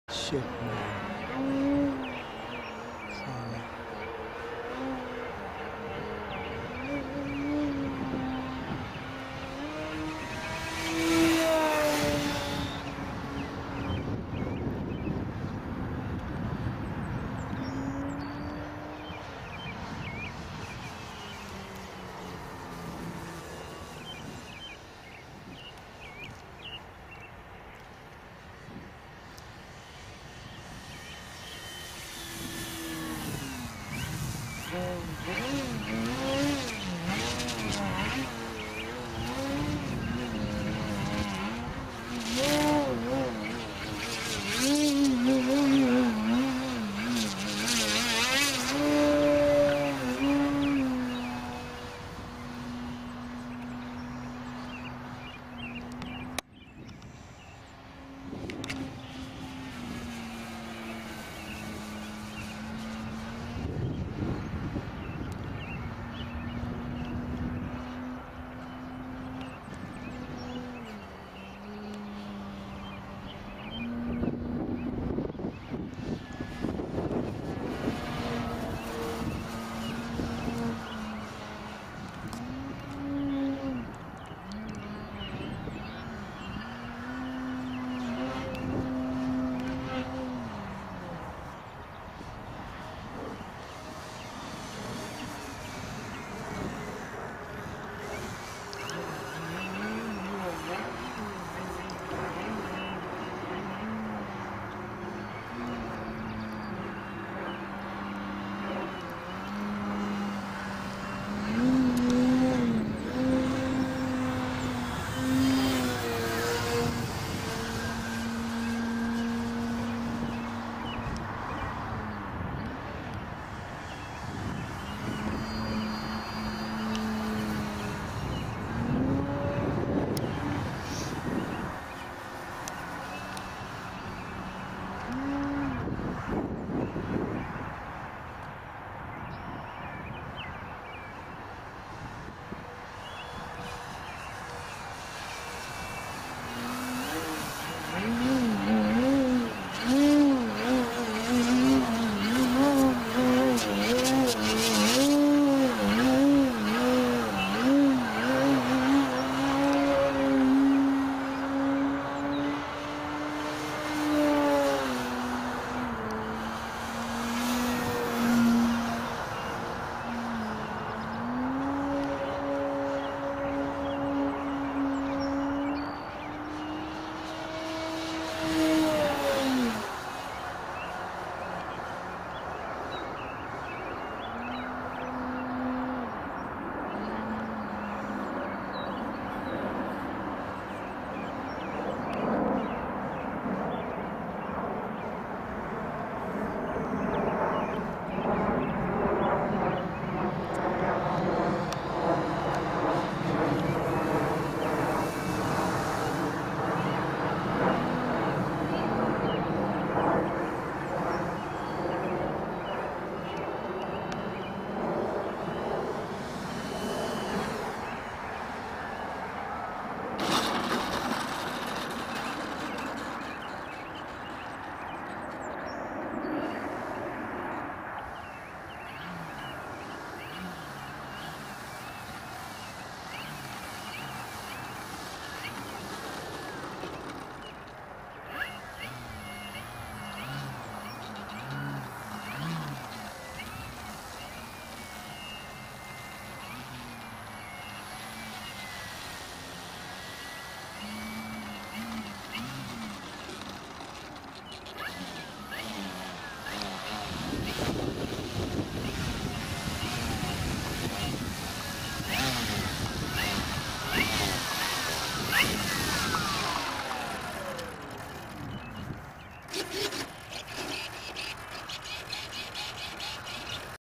E-Flite Beast 60E - Hover / Harrier Practice
E-flite's new Beast 60E is THE best bi-plane I've ever flown... Amazing 3D flight, hover, harrier, knife-edge, everything!! Keep an eye for more videos after I get used to this big beast!!!